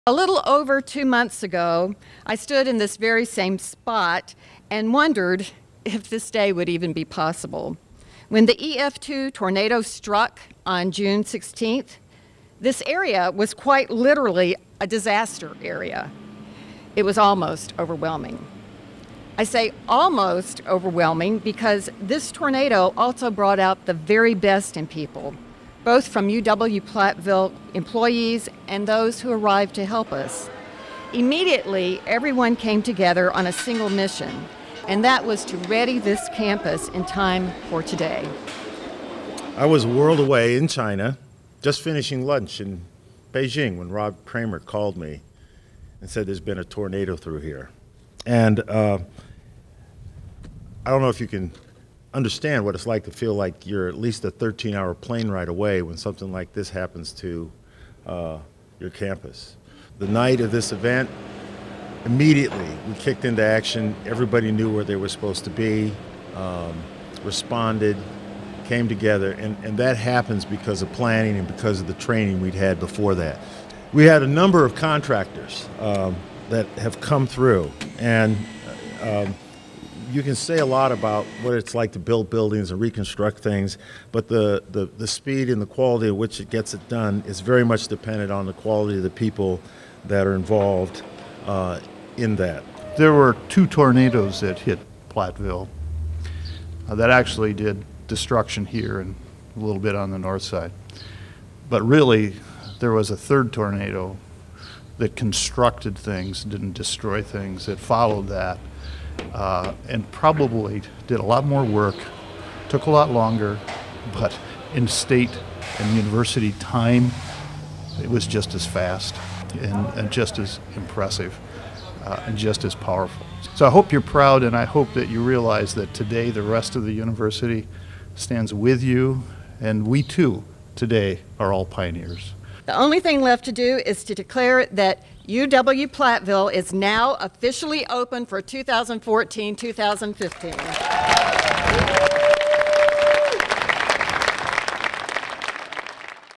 UW-Platteville Officially Re-Opens Campus After Tornado
Just over two months after a tornado ripped through UW-Platteville, students moved in and campus was officially re-opened for the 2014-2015 academic year.